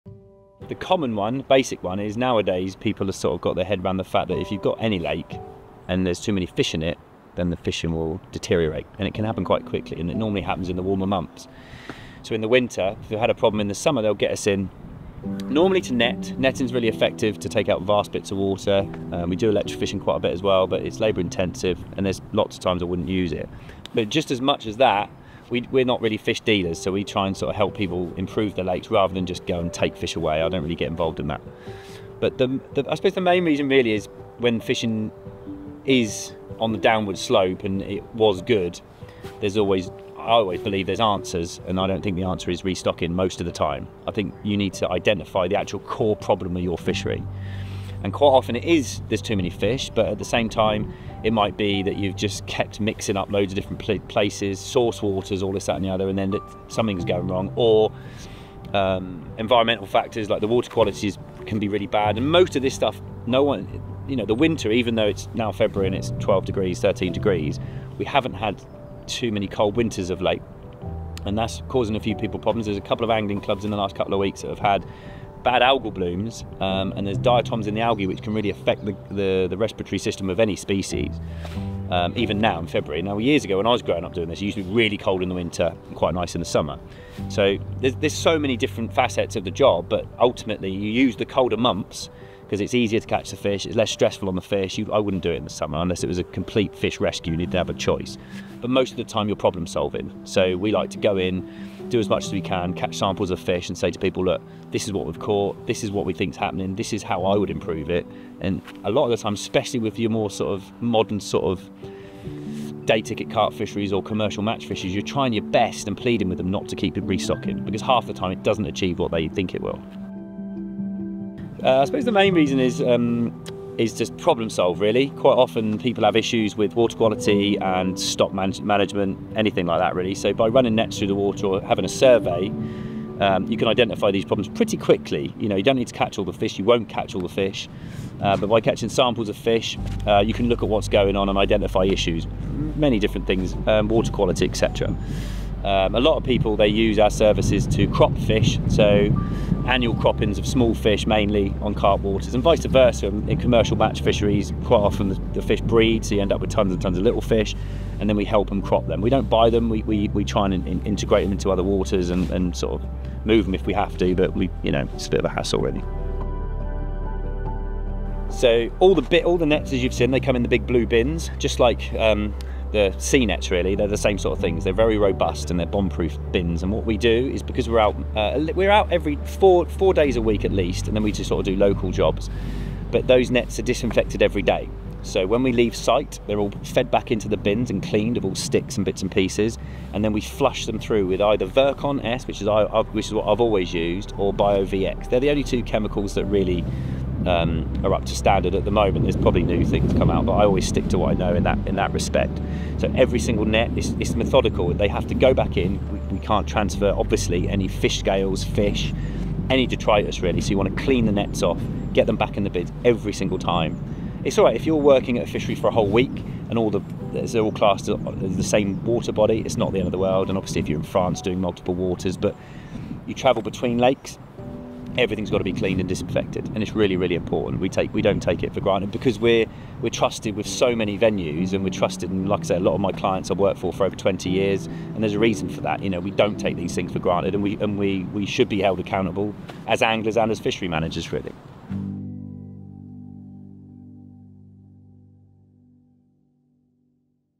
Fish with Carl.
Managing fisheries effectively. In our modern fishery management practice we must realise that the climate is unpredictable.
Heat, flooding and storms lead to many issues that those involved in the successful  management of waterways need to take seriously And adapt.
For the full film follow @fishwithcarl